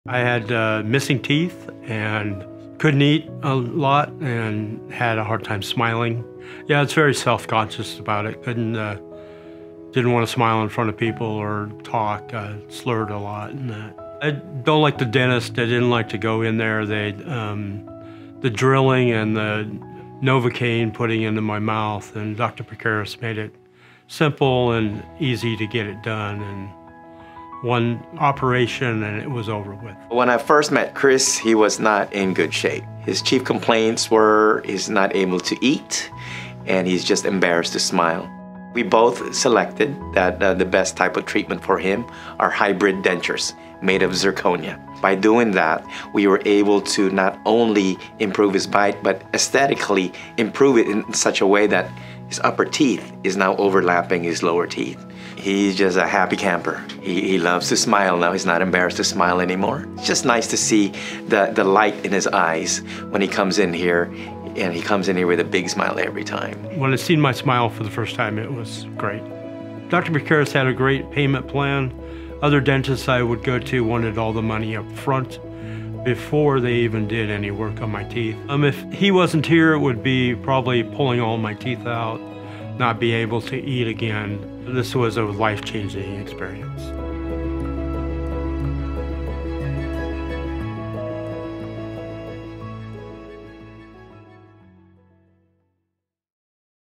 My Missing Teeth Made Eating & Smiling Difficult, I Now Love To Smile Thanks To Hybrid Dentures
Advanced Dental Implant Center in Central Coast, CA
Offices in Santa Maria, CA and Templeton CA.